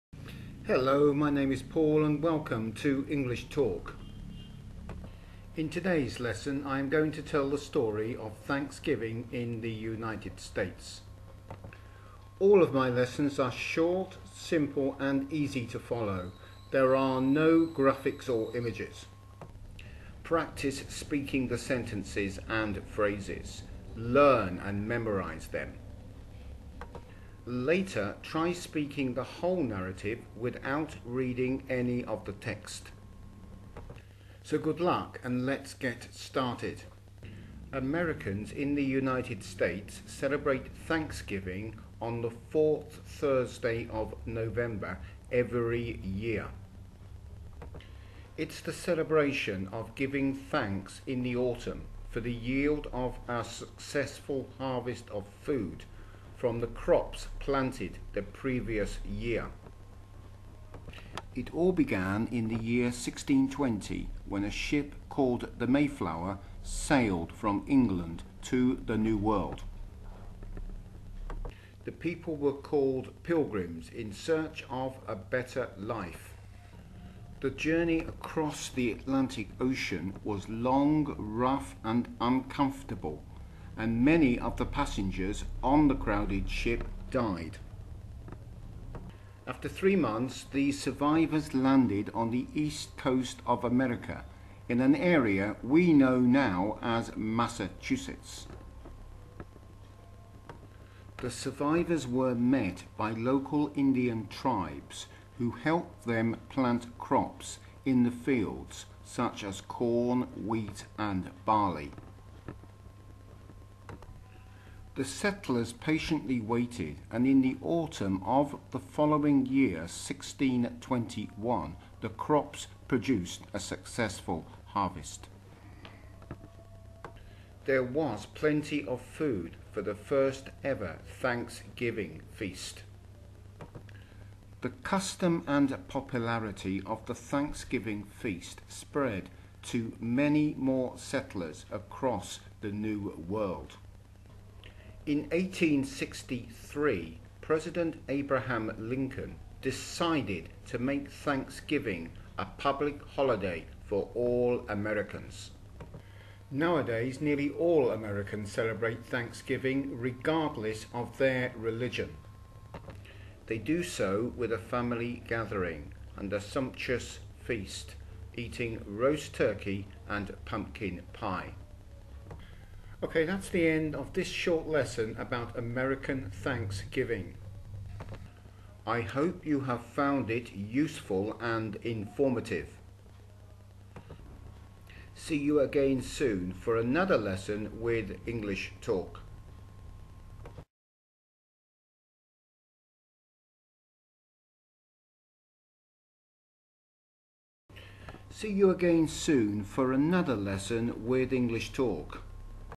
englishtalk thanksgiving
Introduction to the Thanksgiving celebration in the United States of America.  Ideal as a narrative for anyone learning English.
First task is to listen to the text without reading it and write it down.
Check for mistakes afterwards.
Write the basic narrative yourself as a writing exercise from memory.
Then practice speaking the narrative from memory without reading the text.
This gives you excellent opportunities to develop your English language speaking skills.